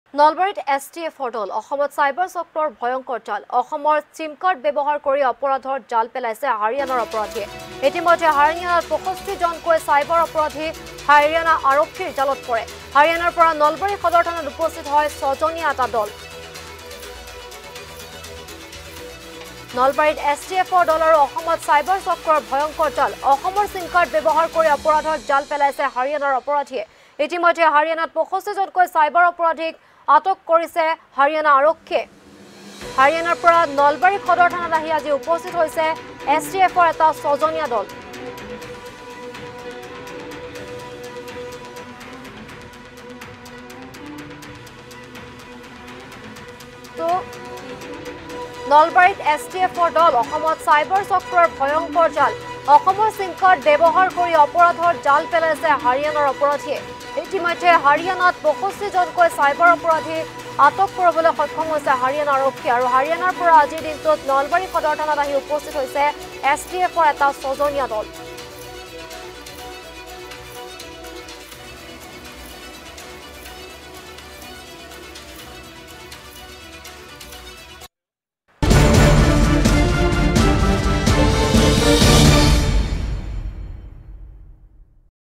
অসমত চাইবাৰ চক্ৰৰ ভয়ংকৰ জাল
🛑অসমৰ চিমকাৰ্ড ব্যৱহাৰ হাৰিয়ানাৰ অপৰাধীৰ
🛑হাৰিয়ানাত আৰক্ষীৰ জালত  ৬৫ জনকৈ চাইবাৰ অপৰাধী